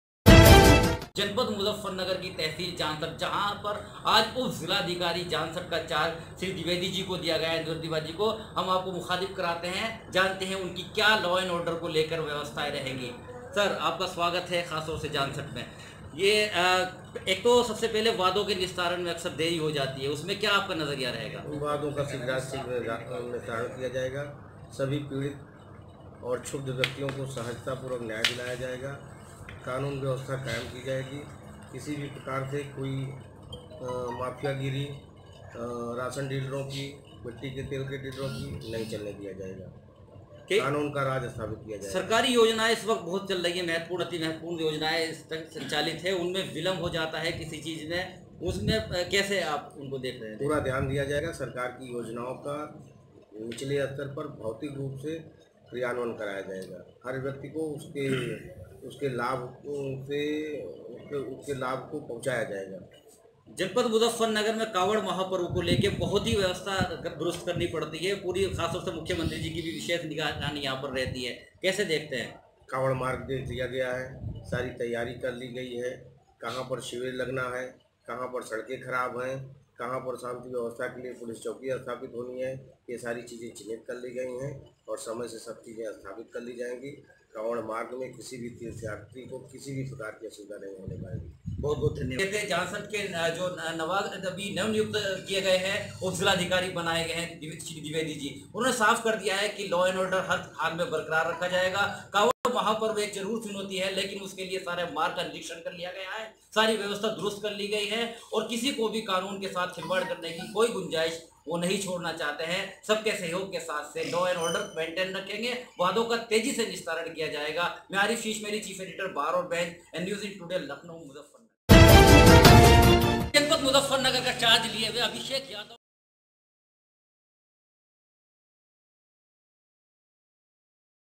कानून का राज बरकरार रहेगा, सरकारी योजनाओं का पात्रों को मिलेगा भरपूर लाभ कावड़ : एसडीएम दिवेदी
मुजफ्फरनगर ..जानसठ 10 जून। तहसील जानसठ के नवागत उपजिलाधिकारी इंद्रकांत द्विवेदी ने तहसील में कार्यभार संभाल लिया है। आज प्रथम दिन बार और बेंच के चीफ एडिटर आरिफ शीश महली एडवोकेट से खास वार्ता करते हुए उन्होंने दावा किया कि वादों का निस्तारण तेजी से होगा और कानून का राज कायम होगा ।उन्होंने यह भी कहा कि सरकारी योजनाओं का लाभ हर व्यक्ति को मिले इसके लिए कोई कोर कसर बाकी नहीं छोड़ेंगे। एक प्रश्न के जवाब में उन्होंने कहा कि कावड़ महापर्व को लेकर सभी तैयारियां पूर्ण कर ली गई हैं, मार्गो का निरीक्षण कर लिया गया है। शिविर लगने की स्थिति का जायजा लिया गया है ।प्रशासन पूरी तरह से मुस्तैद है, जनता के सहयोग से क्षेत्र में कानून का राज और सभी महापर्व सकुशल संपन्न कराए जाएंगे। एक प्रश्न के जवाब में उन्होंने यह भी कहा कि क्षेत्र में चाहे राशन की समस्या हो या आधार कार्ड की किसी भी समस्या को गंभीरता से लिया जाएगा और शिकायत कर्ताओं को इधर उधर भटकने नहीं दिया जाएगा। उन्होंने कहा कि इन दिनों स्कूल कॉलेज खुले हुए हैं। छात्र-छात्राओं को किसी प्रमाण पत्र बनवाने में दिक्कत ना आए इसके लिए भी तेजी से कार्य कराए जाएगा।
 विदित हो जानसठ के लोकप्रिय रहे एसडीएम विजय कुमार के अवकाश पर चले जाने के बाद जानसठ तहसील का कार्यभार कुछ दिनों के लिए अजय कुमार  को दिया गया था जिन्होंने तेजी से कार्य करते हुए क्षेत्र में बहुत सी समस्याओं का समाधान किया ,वहीं अब जिलाधिकारी अजय शंकर पांडे ने अजय कुमार को खतौली का उप जिलाधिकारी बनाया है तो वहां के उपजिलाधिकारी इंद्रकांत द्विवेदी को जानसठ तहसील की जिम्मेदारी सौंपी है।
इससे पूर्व आज प्रेस क्लब का प्रतिनिधिमंडल भी उपजिलाधिकारी से मिला और शिष्टाचार भेंट में एक दूसरे के सहयोग की बात कही गई। इस मौके पर पंजाब केसरी से मोहम्मद अहसान, राष्ट्रीय सहारा से अनुज सैनी, दैनिक भास्कर और प्रांत बुलेटिन से मेहराजुद्दीन प्रेस क्लब के मुख्य संरक्षक ने आर्य शीश महल ई के नेतृत्व में शिष्टाचार भेंट की।